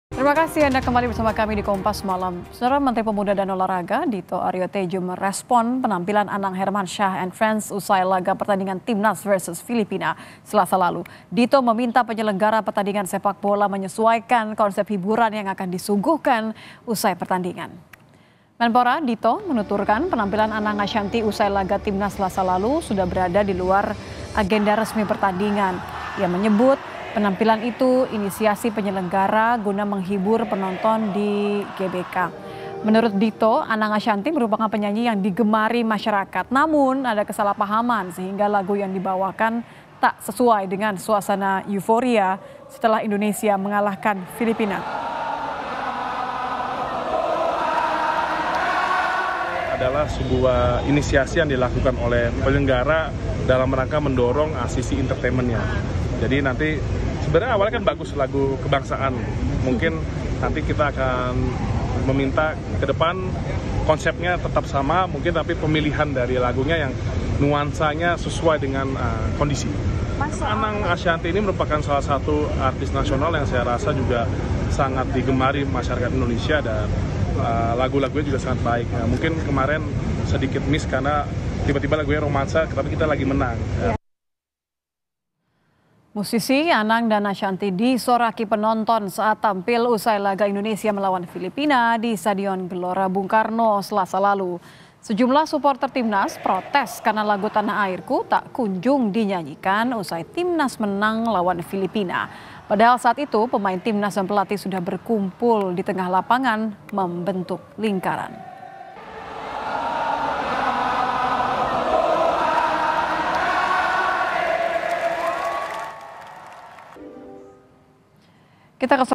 Anang-Ashanty Disoraki Suporter Timnas Indonesia, ini Respons Menpora Dito Ariotedjo!
KOMPAS.TV - Menteri Pemuda dan Olahraga (Menpora), Dito Ariotedjo merespons penampilan Anang Hermansyah dan kawan-kawan usai laga pertandingan timnas versus Filipina pada Selasa (11/6) lalu.

Dito meminta penyelenggara pertandingan sepak bola, menyesuaikan konsep hiburan yang akan disuguhkan usai pertandingan.

Menpora Dito menuturkan, penampilan Anang-Ashanti seusai laga Timnas lalu, sudah berada di luar agenda resmi pertandingan.

Ia menyebut penampilan itu inisiasi penyelenggara guna menghibur penonton di SUGBK.

Menurut Dito, Anang Ashanti merupakan penyanyi yang digemari masyarakat.

Namun, ada kesalahpahaman; sehingga lagu yang dibawakan tak sesuai dengan suasana euforia setelah Indonesia mengalahkan Filipina.